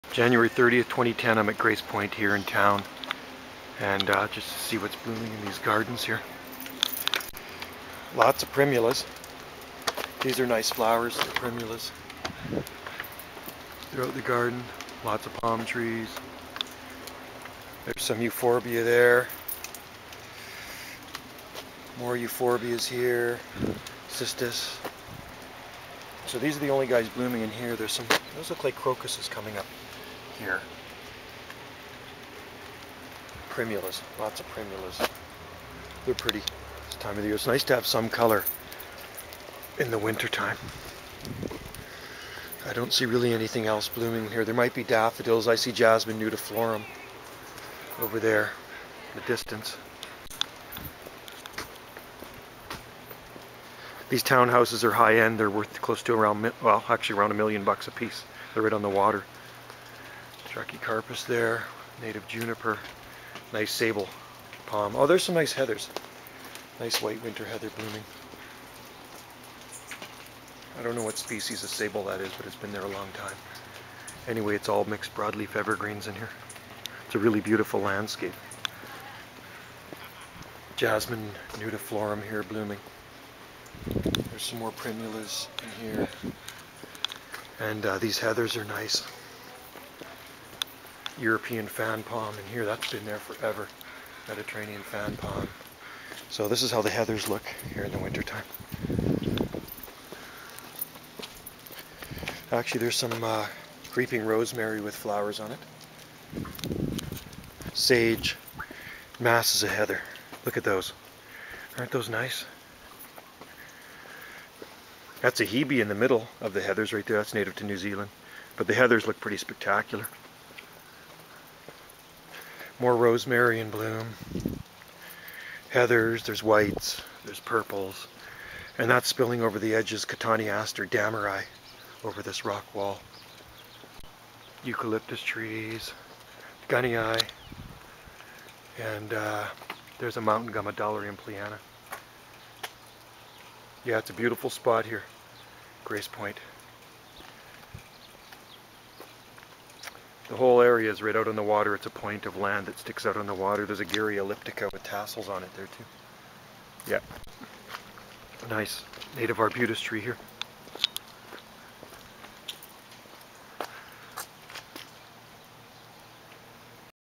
Grace Point Plants on Salt Spring Island Part 1
Grace Point is located in our main town. They seem to grow every plant under the sun in this mild Seaside microclimate. Jan. 30, 2010